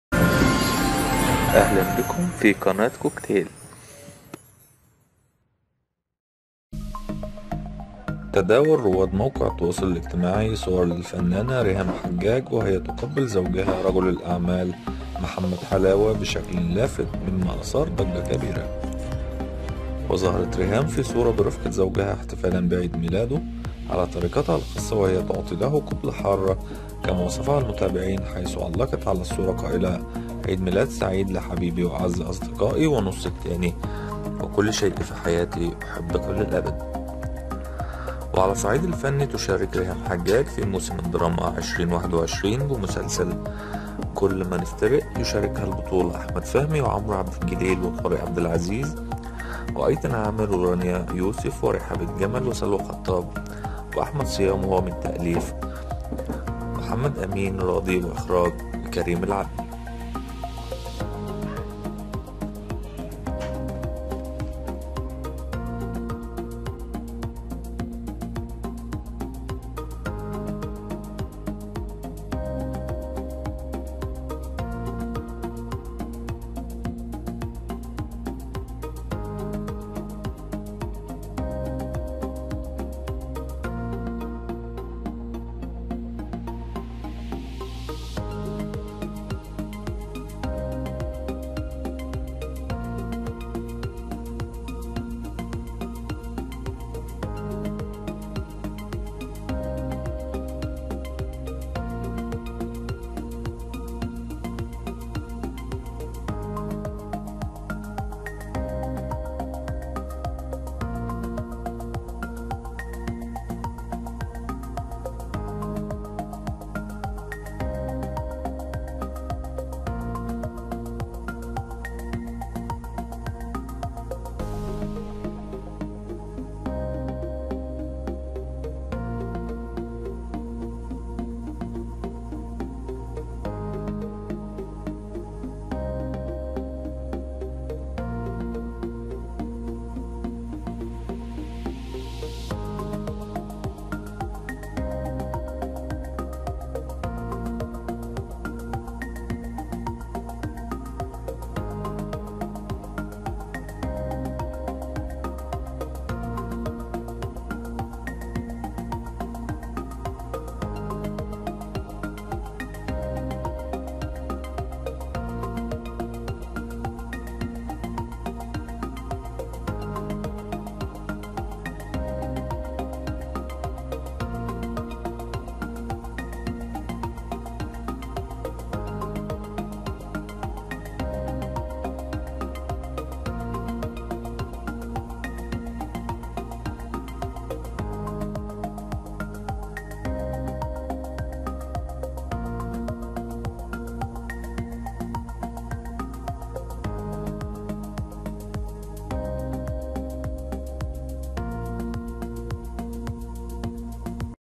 "قبلة حارة" من ريهام حجاج لزوجها "حلاوة" تشعل السوشيال ميديا
وظهرت "ريهام" في الصورة برفقة زوجها احتفالا بعيد ميلاد على طريقتها الخاصة، وهي تعطي له "قبلة حارة" كما وصفها المتابعين، حيث علقت على الصورة قائلة: "عيد ميلاد سعيد لحبيبي وأعز أصدقائي ونصفي الثاني، وكل شيء في حياتي.. أحبك إلى الأبد".